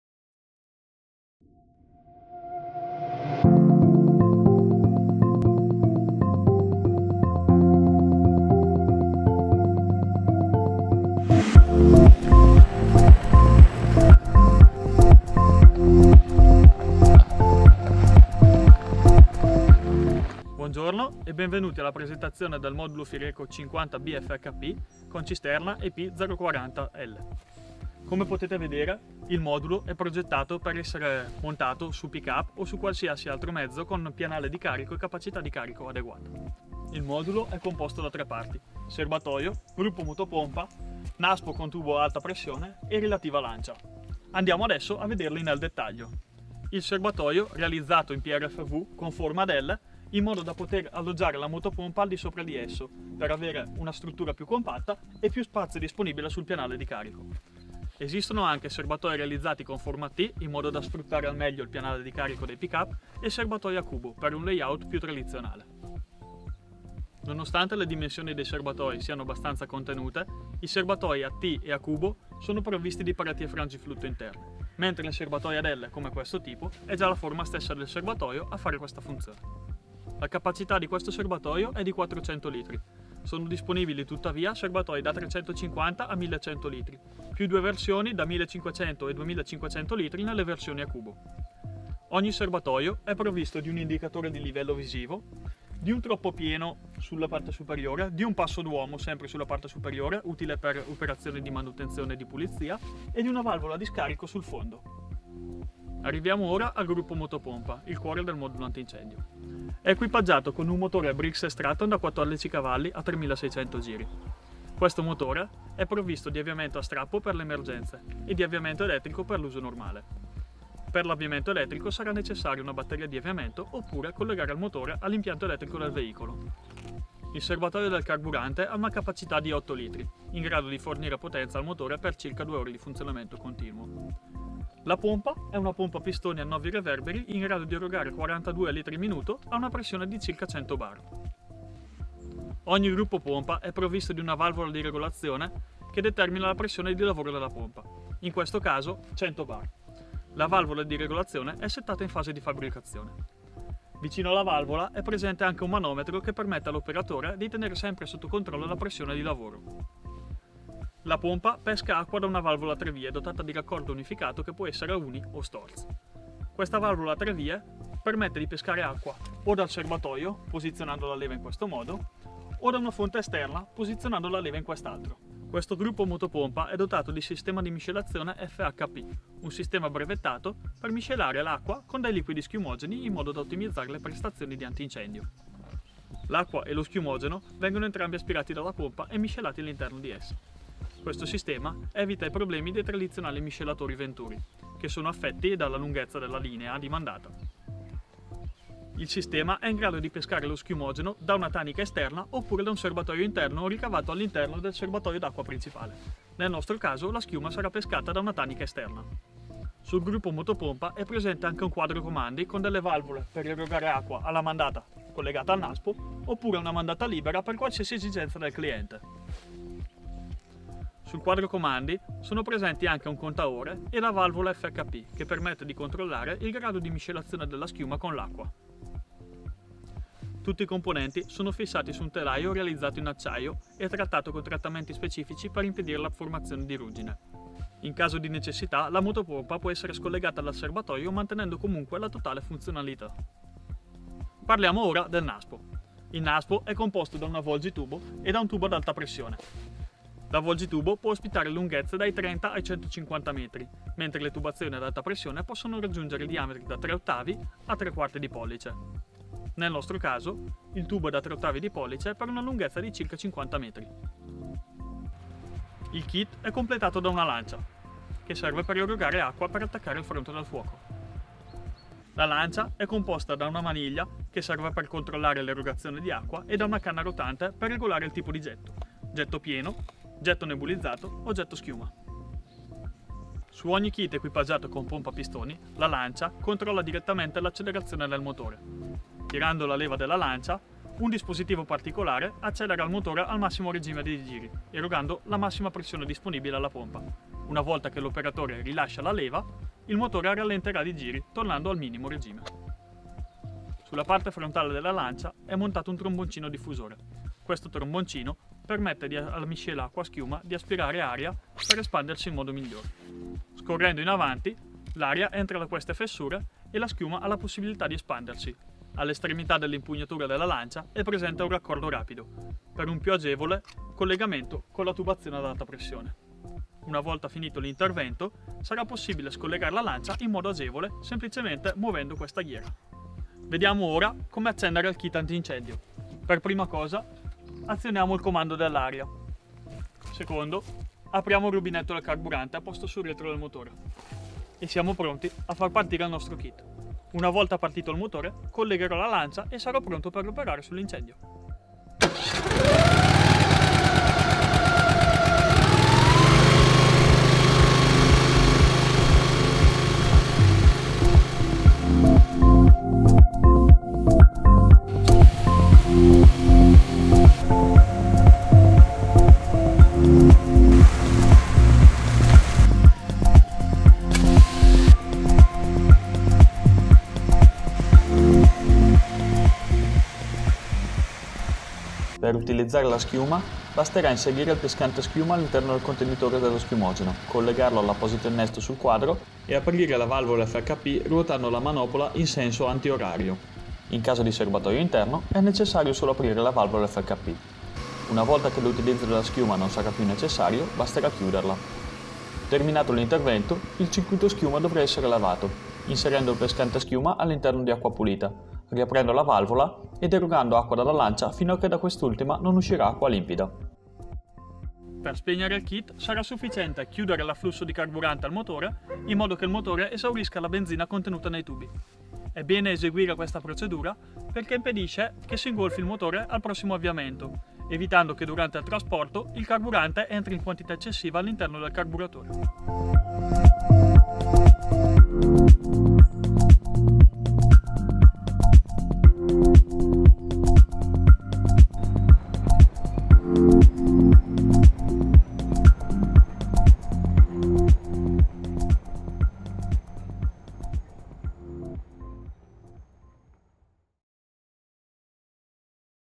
Kit anti-incendie Haute Pression pour pick-up Sapeur-pompier
Les kits incendie Fireco sont conçus pour lutter contre les feux de forêt.
Kits d'extinction multi-fonctions conçus pour des véhicules de type pick-up

Kits de haute pression fiables avec des composants de haute qualité dans un design compact avec un centre de gravité très bas dans le réservoir qui permet d'adapter les structures porte-outils afin de disposer d'un véhicule multifonctionnel. Les kits d'extinction haute pression Fireco sont adaptés à tous les modèles de véhicules 4x4.